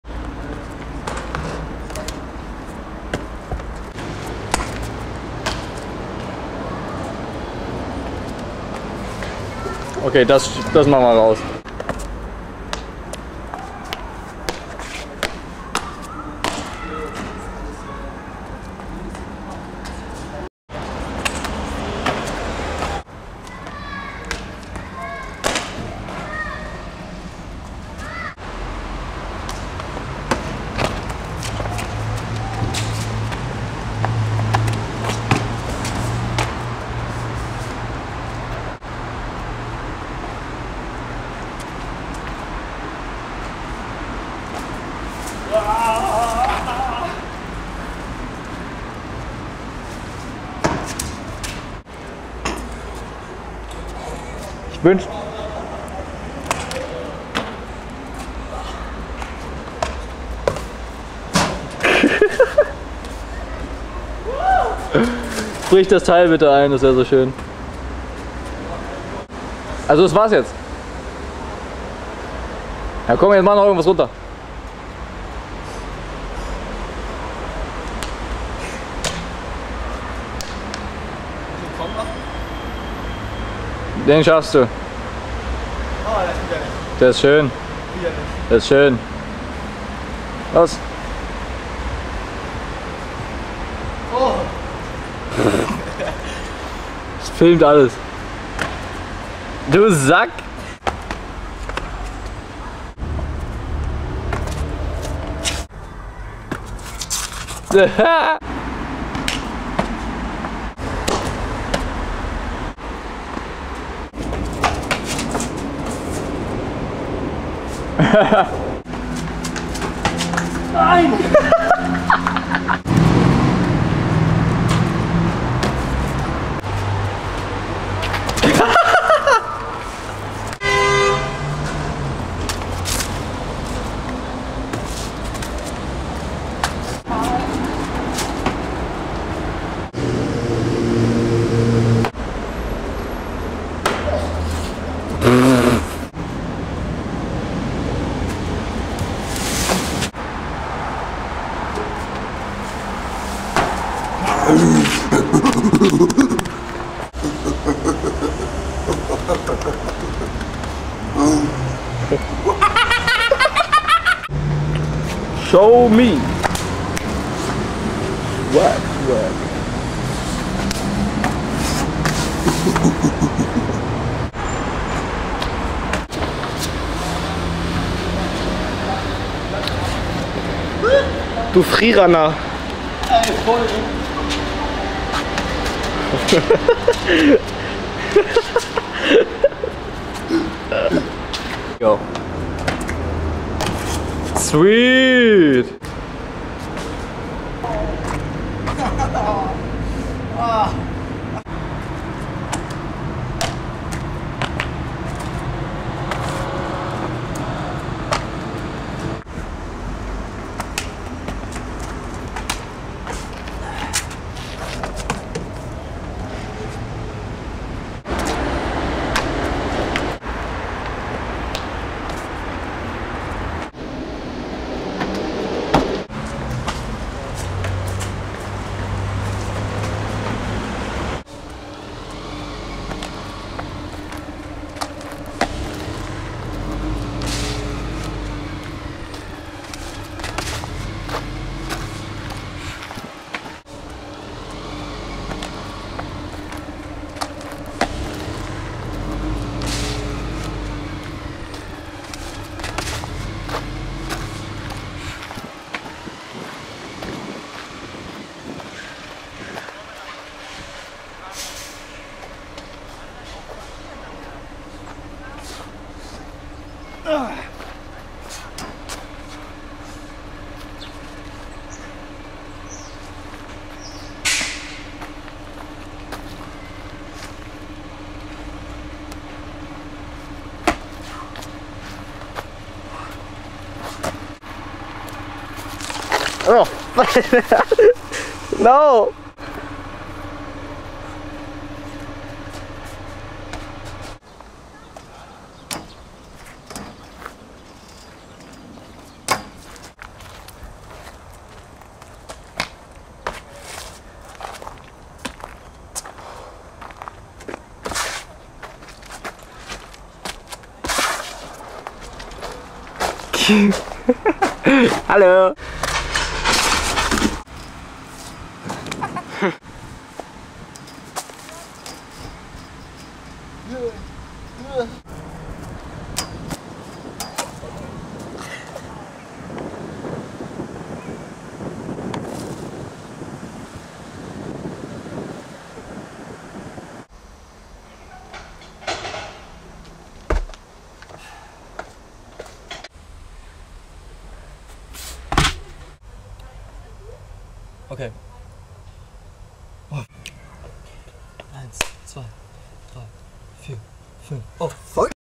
Training Session - Jeele und Simon
Fußgänger Jeele und Simon beim Training an zwei seeeeeeehr heißen Tagen im Sommer 2016.